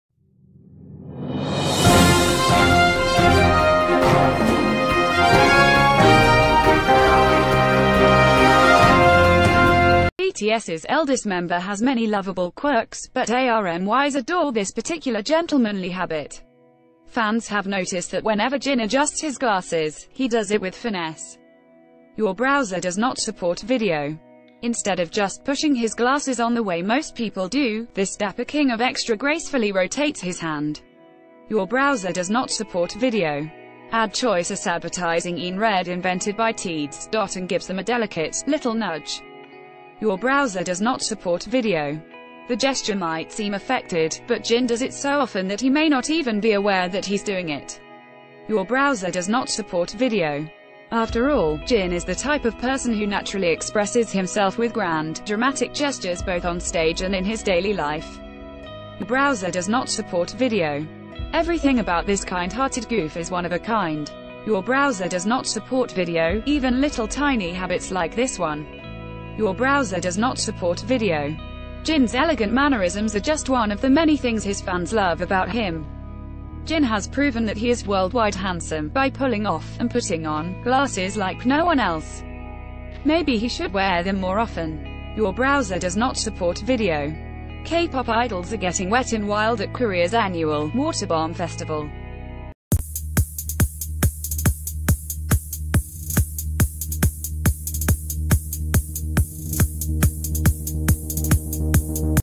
BTS Fans Have Found Another One Of Jin’s Quirky Habits
BTS‘s eldest member has many lovable quirks, but ARMYs adore this particular gentlemanly habit. Fans...